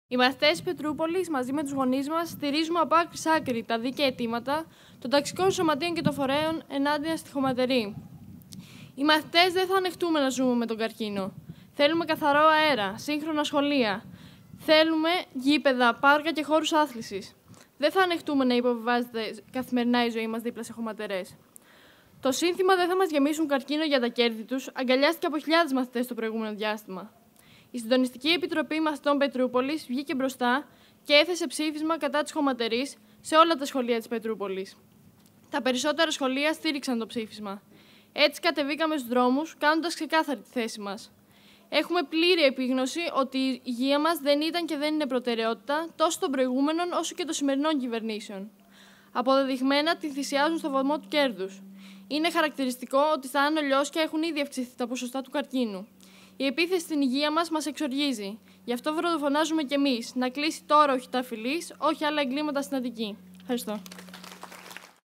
ΠΑΡΕΜΒΑΣΗ ΣΤ. ΚΟΝΤΑΡΓΥΡΗ, ΣΥΝΤΟΝΙΣΤΙΚΟ ΜΑΘΗΤΩΝ ΠΕΤΡΟΥΠΟΛΗΣ
Σε συνάντηση με 35 φορείς της Δυτικής Αθήνας και Δυτικής Αττικής μίλησε σήμερα Παρασκευή 5 Ιούνη, Παγκόσμια Ημέρα Περιβάλλοντος, ο ΓΓ της ΚΕ του ΚΚΕ Δημήτρης Κουτσούμπας.  Η συνάντηση πραγματοποιήθηκε στο Πάρκο Τρίτση, εμβληματικό των προσπαθειών και του αγώνα τόσο των κατοίκων της Δυτικής Αττικής όσο και του ΚΚΕ για ανοιχτούς ελεύθερους χώρους, ποιότητα ζωής, προστασία του περιβάλλοντος.

Στο βίντεο μπορείτε να δείτε την παρέμβαση της Σταυρούλας Κονταργύρη, απ' το Συντονιστικό Μαθητών Πετρούπολης.